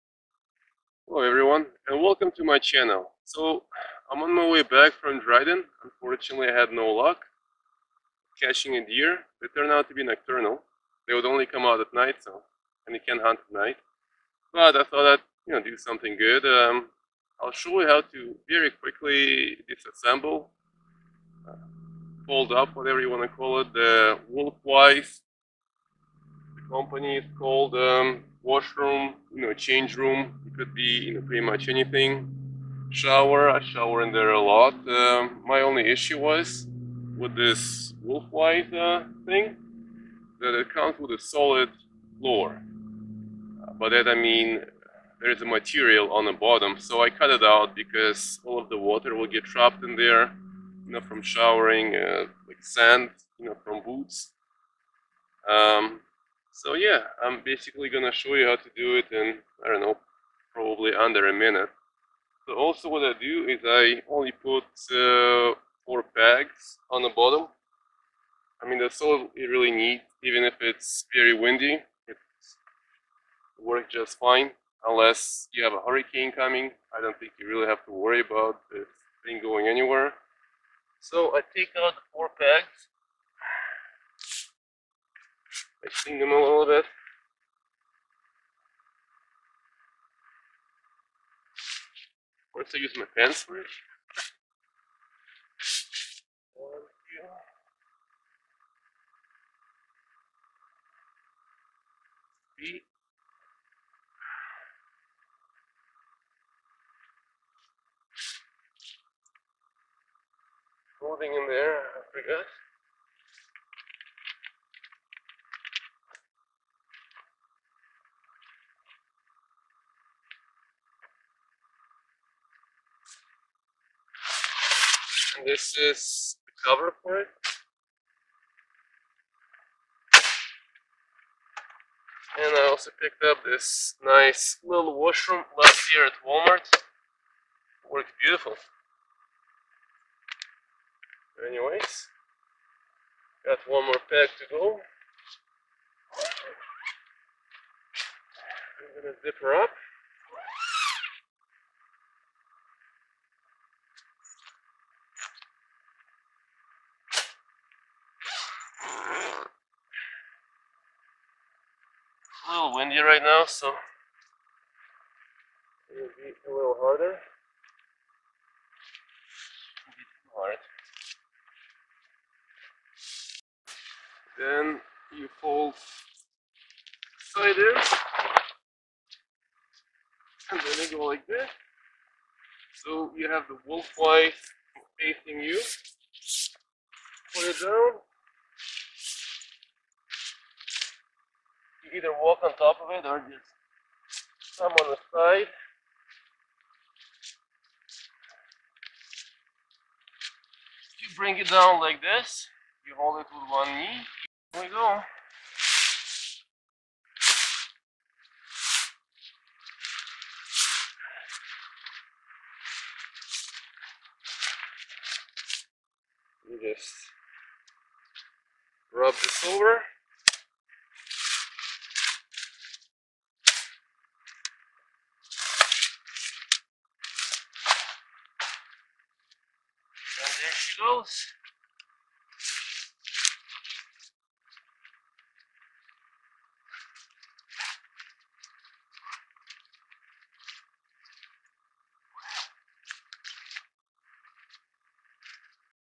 How to disassemble a Wolfwise changing tent in 30 seconds
In this video I'm explaining how to quickly disassemble a Wolfwise changing tent.